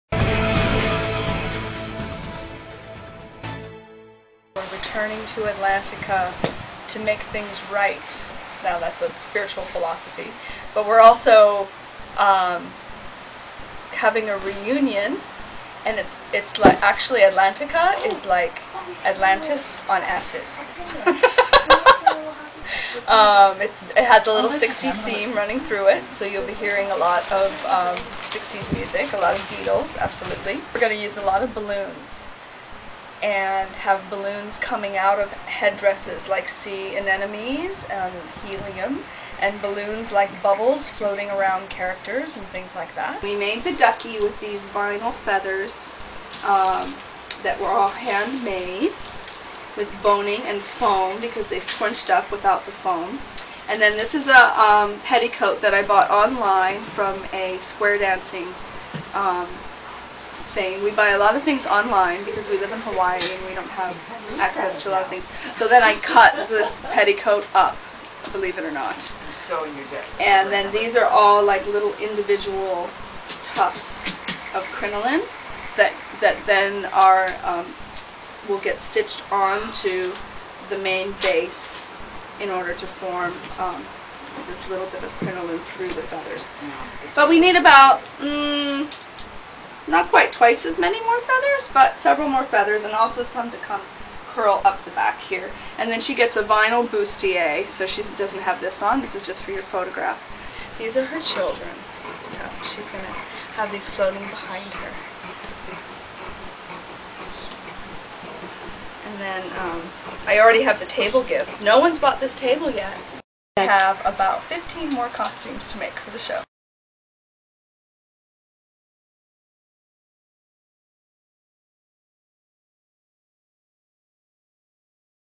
Iona Atlantika
Explore the deep blue world of the lost city in IONA Contemporary Dance Theatre's fundraising event.